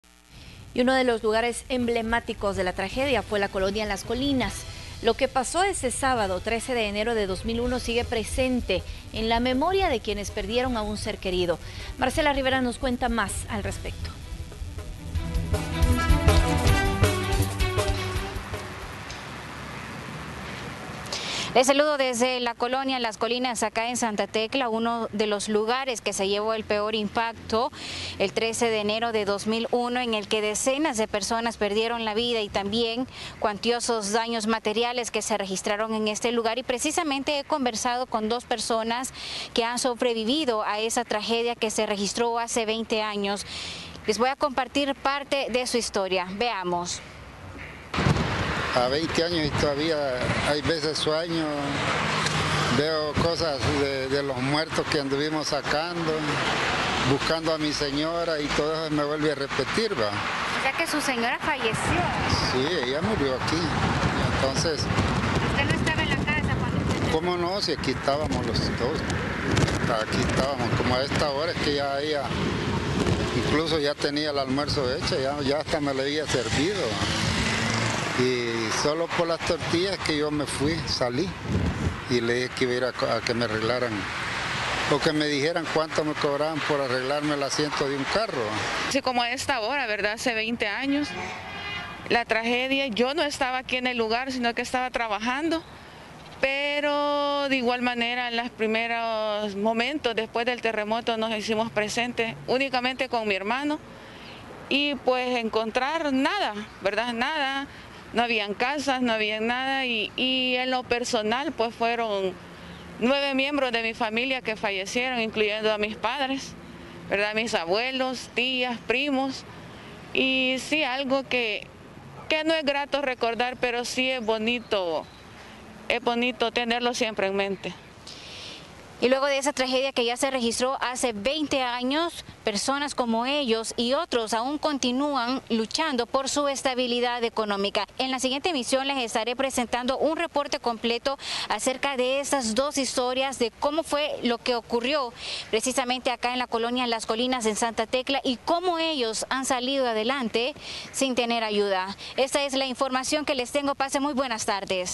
La colonia Las Colinas fue uno de los lugares emblemáticos del terremoto del 2001
Santa Tecla fue uno de los lugares que se llevó el peor impacto del terremoto

Lo que pasó ese sábado 13 de enero, sigue presente en la memoria de quienes perdieron a un ser querido